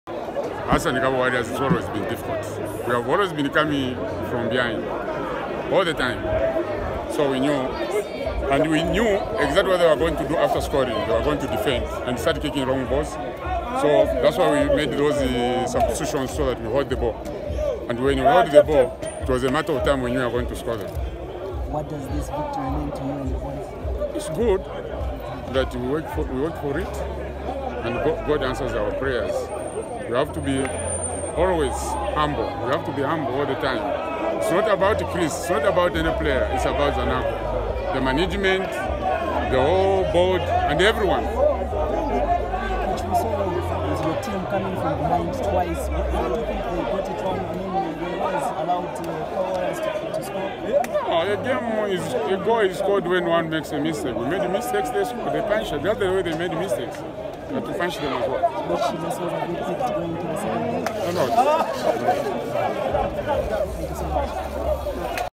2021 ABSA QUARTER FINAL POST MATCH INTERVIEW WITH COACH CHRIS KAUNDA.
2021 ABSA CUP QUARTER-FINAL

FULL-TIME: Zanaco 2-2 Kabwe Warriors

Zanaco qualified to the semi-finals of the 2021 ABSA Cup via a 6-5 score on post-match penalties yesterday 14th March 2021.